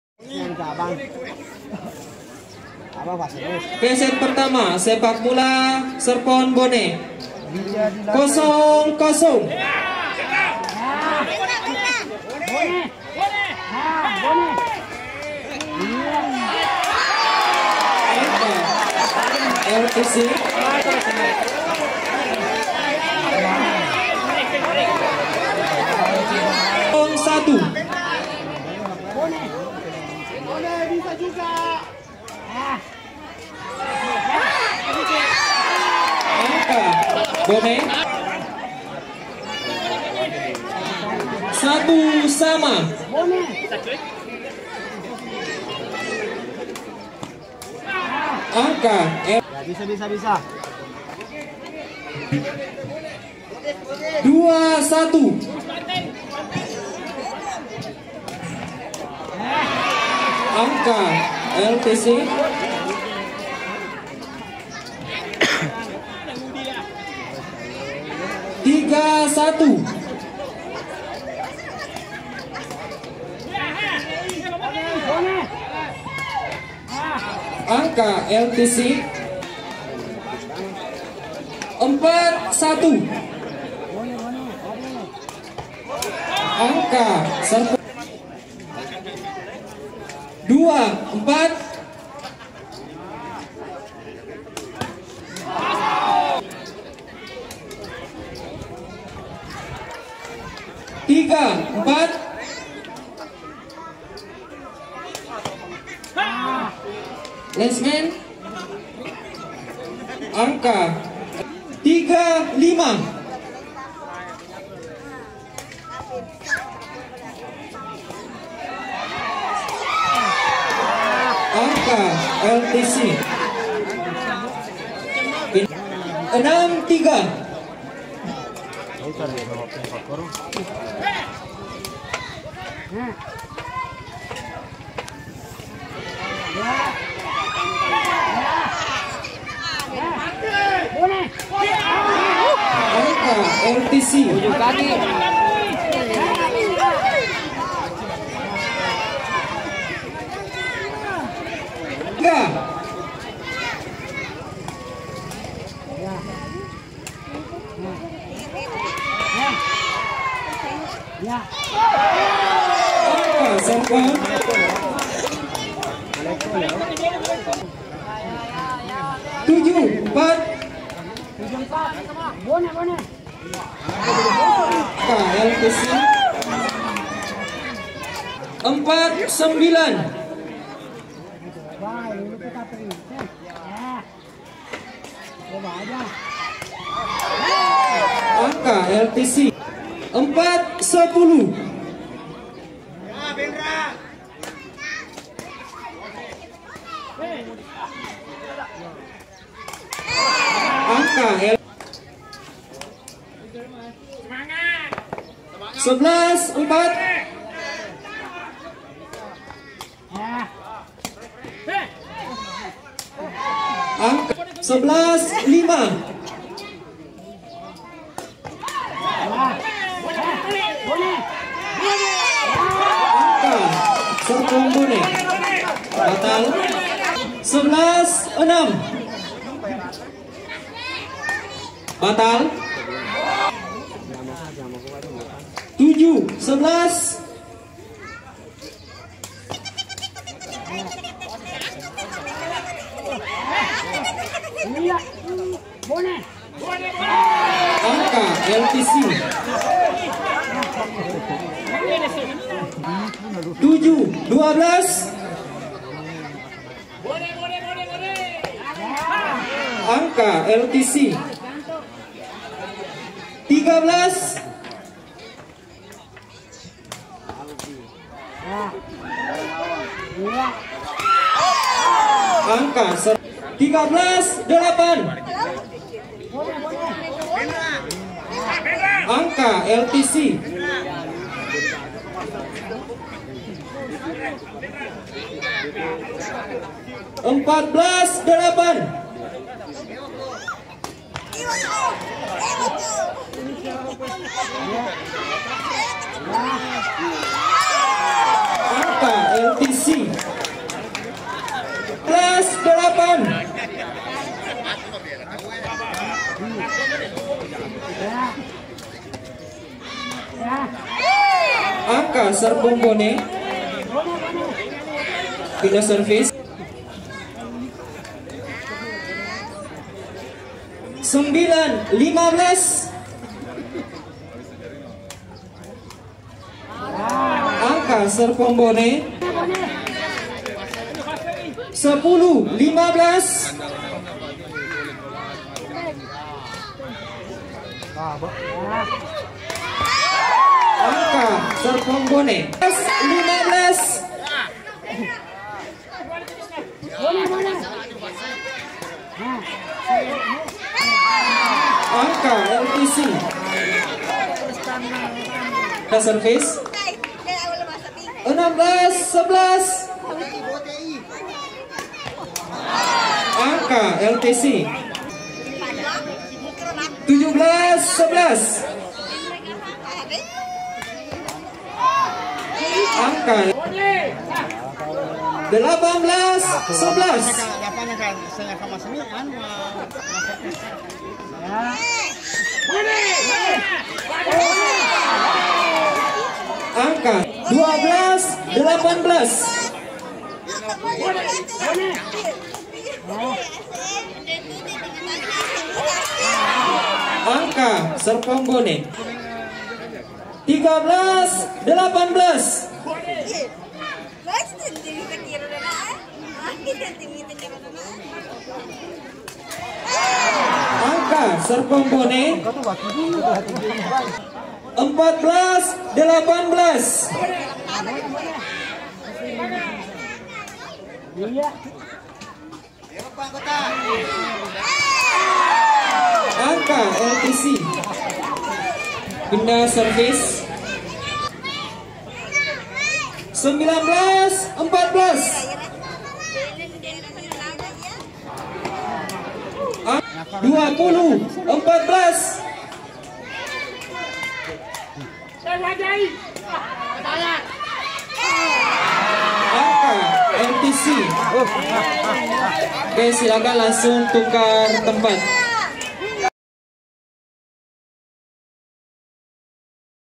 SERPON BONE vs LTC PINRANG (Set 1) SepakTakraw Tarkam Turnamen NEPO CUP II Tahun 2022
SERPON Kab. BONE vs LTC Kab. PINRANG (Set 1) SepakTakraw Tarkam Turnamen NEPO CUP II Tahun 2022

Lokasi: Desa Nepo Kec. Tanasitolo Kab. Wajo Indonesia

Copyright by Sea Games ID